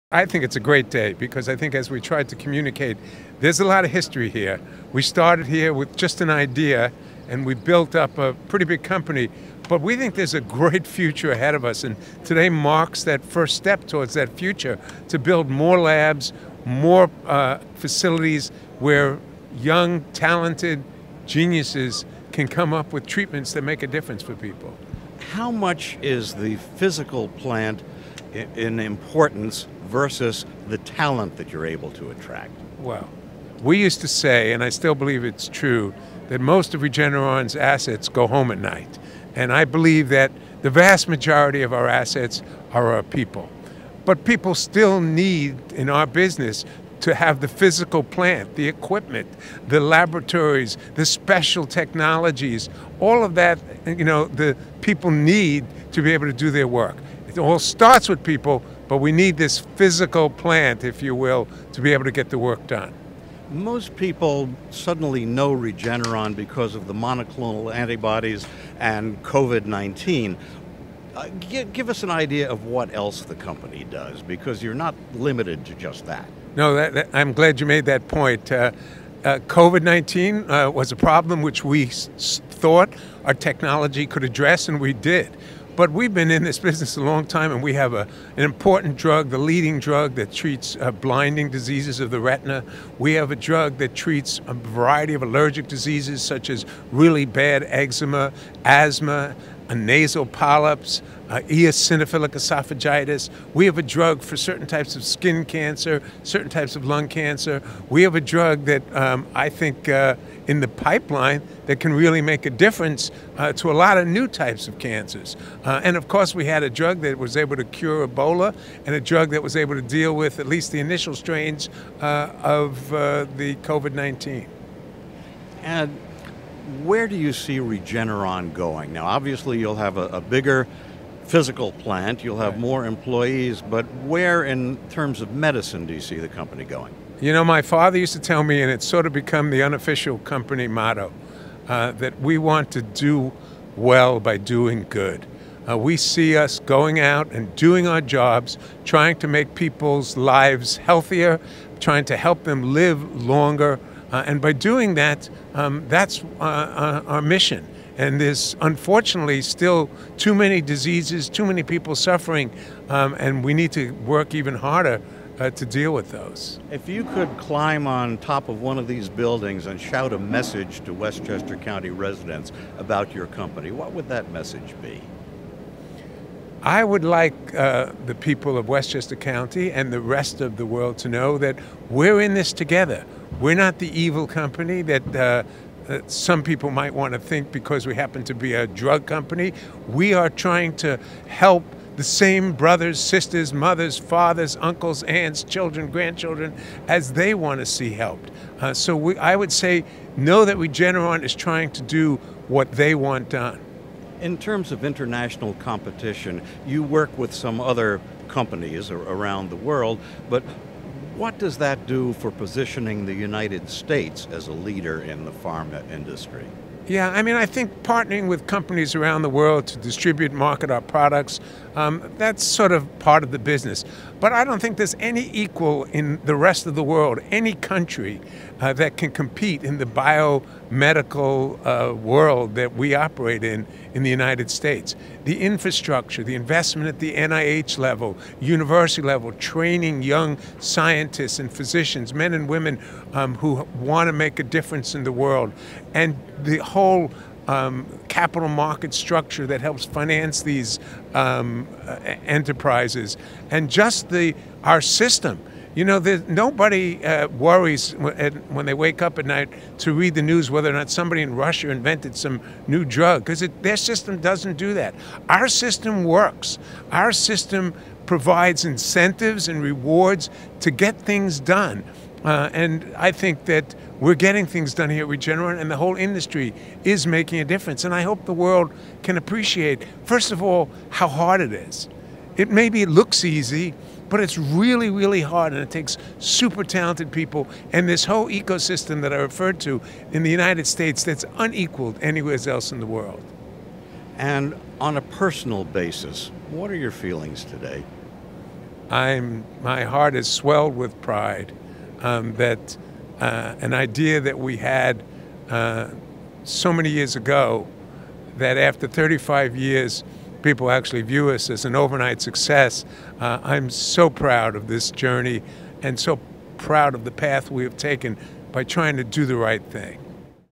Leonard Schleifer
Leonard Schleifer, founder, president and CEO of Regeneron is interviewed by Peter Katz on the occasion of the company's groundbreaking for its $1.8 billion expansion program.,